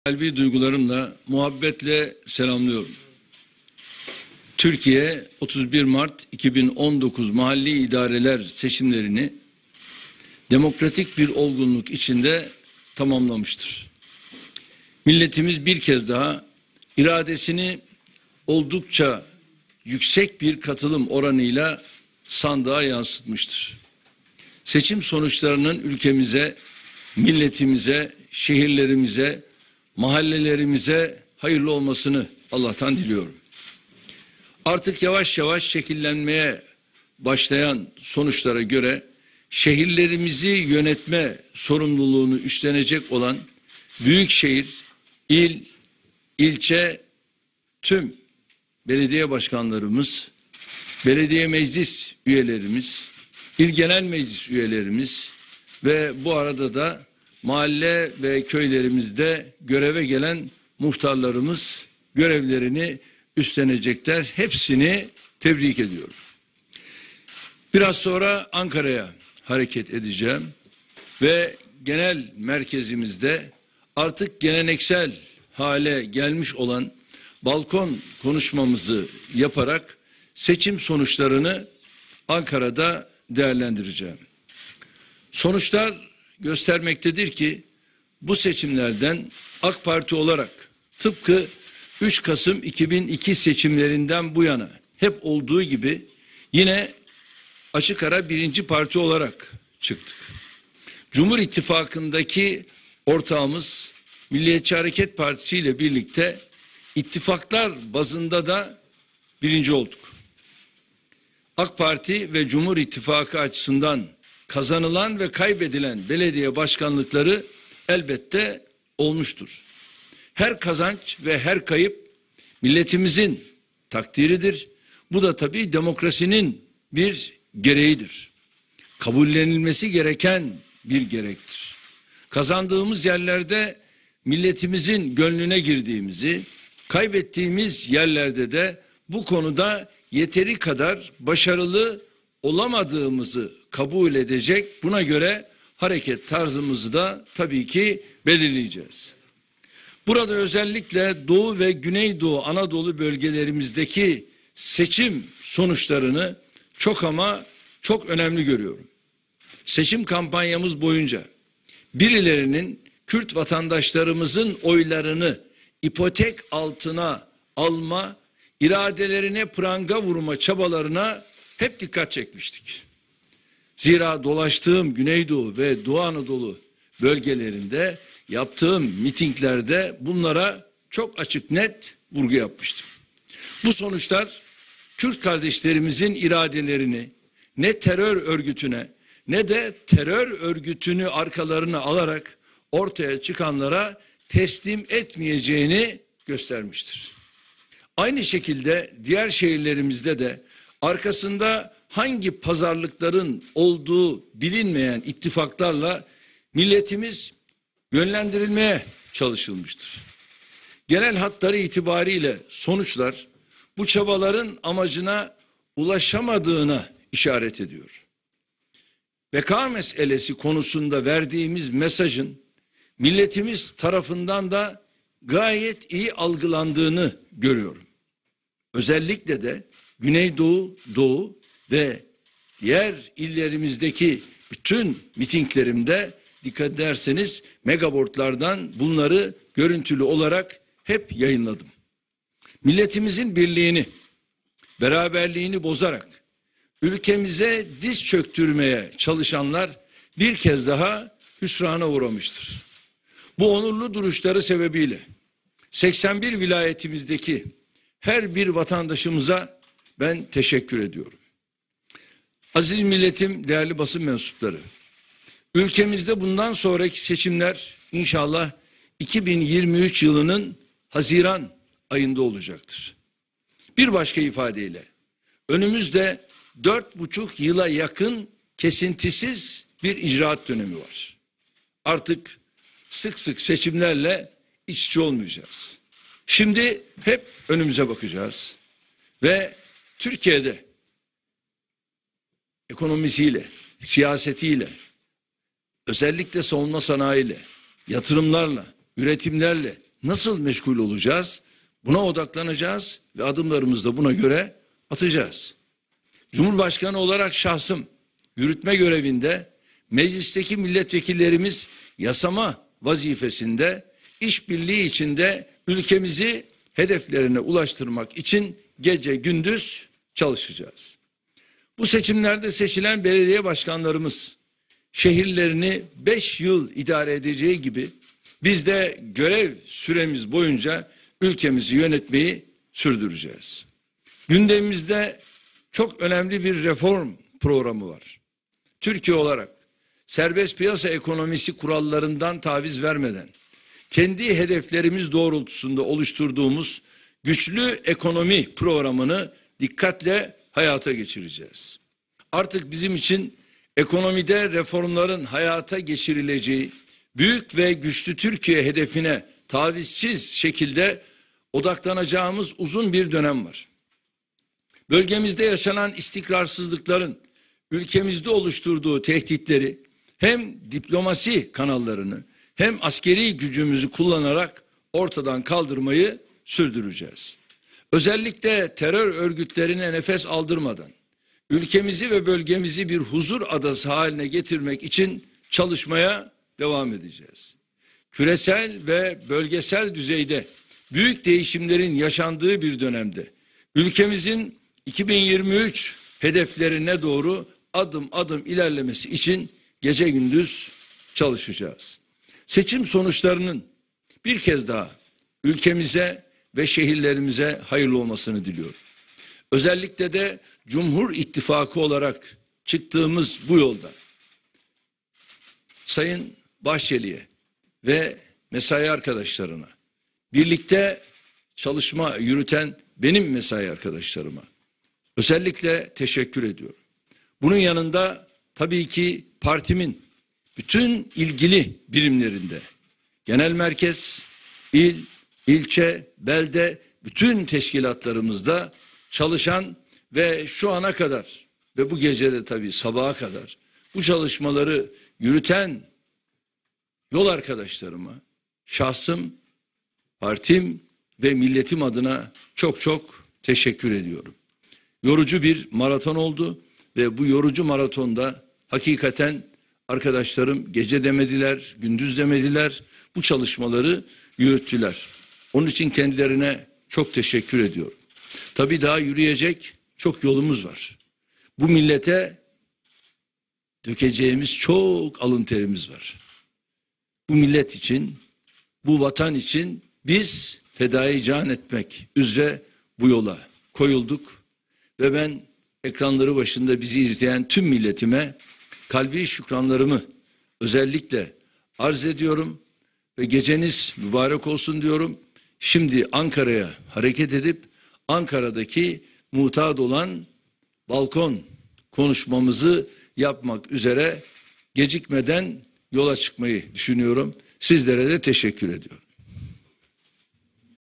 Erdoğan'ın Seçim Akşamı İlk Konuşması: 31.Mart.2019 Belediye Seçimleri Akşamı (Yerel Seçimler)
Sayın Cumhurbaşkanımız Recep Tayyip Erdoğan'ın 31.Mart.2019 Yerel Belediye Seçimlerinin Yapıldığı Akşam İlk Konuşması.